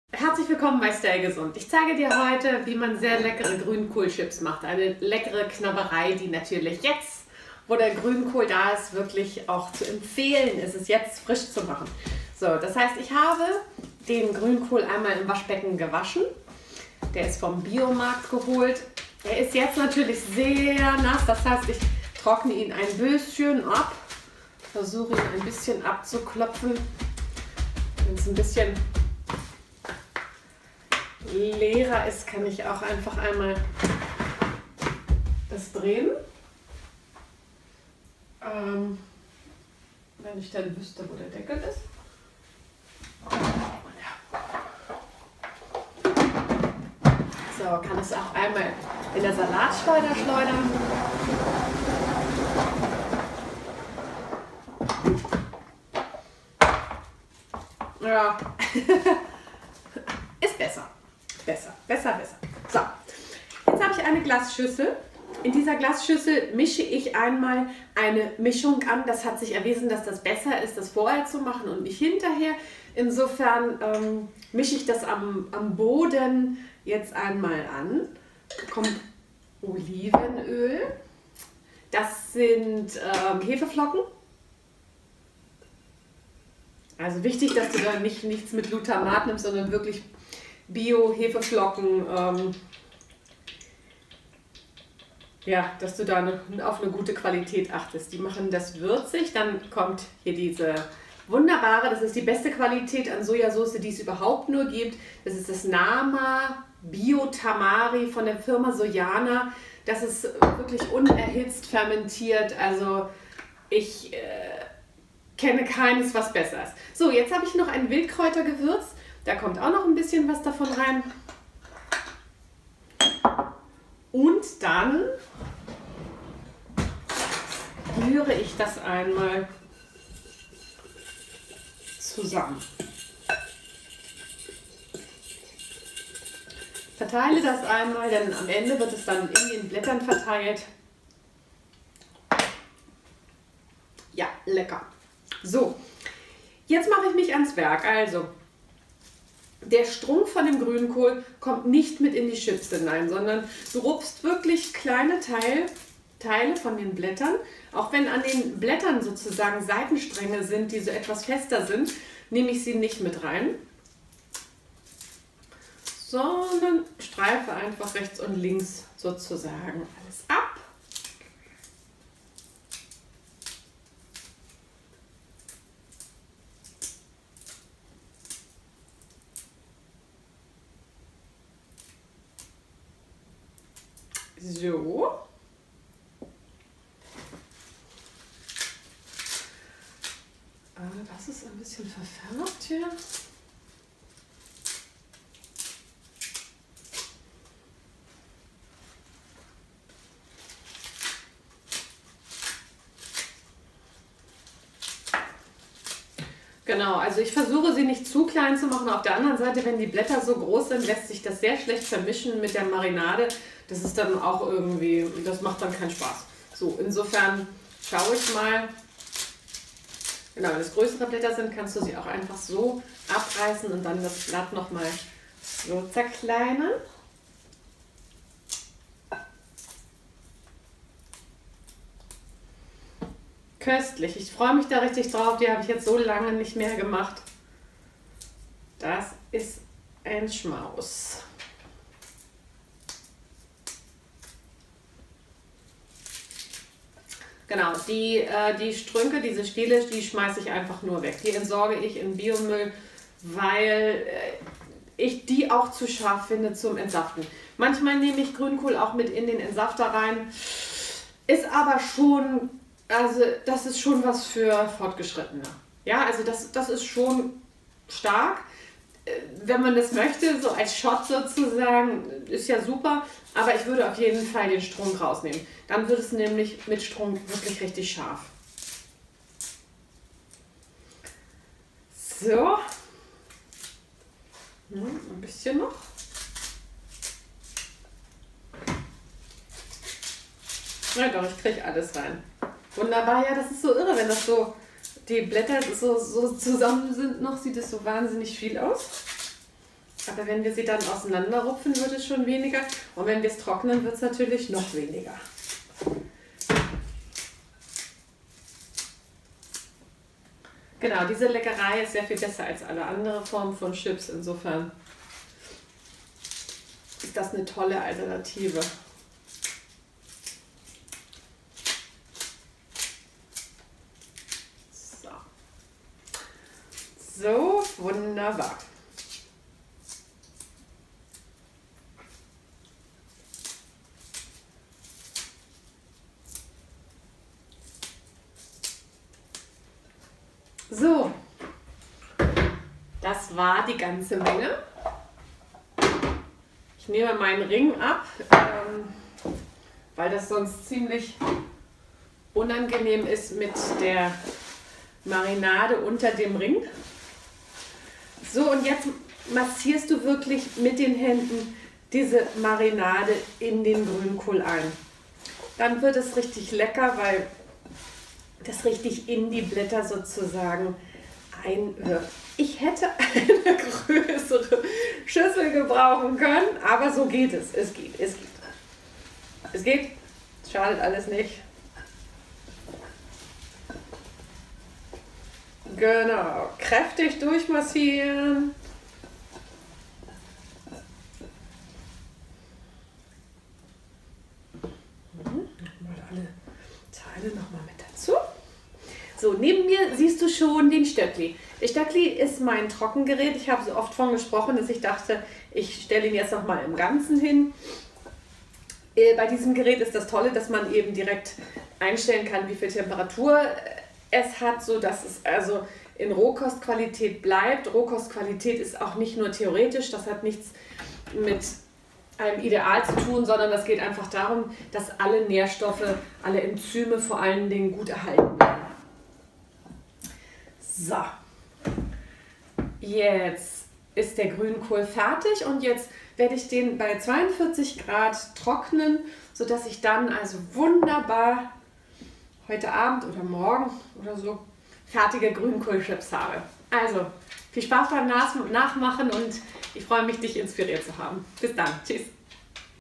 Grünkohlchips selber machen
In diesem Video zeige ich dir, wie du ganz einfach selber Grünkohlchips herstellen kannst. Meine Zutaten:
Sojasoße:
bio-Hefeflocken ohne Gluten
Mein Trockengerät
Bestes Olivenöl - reif geerntet, bio und roh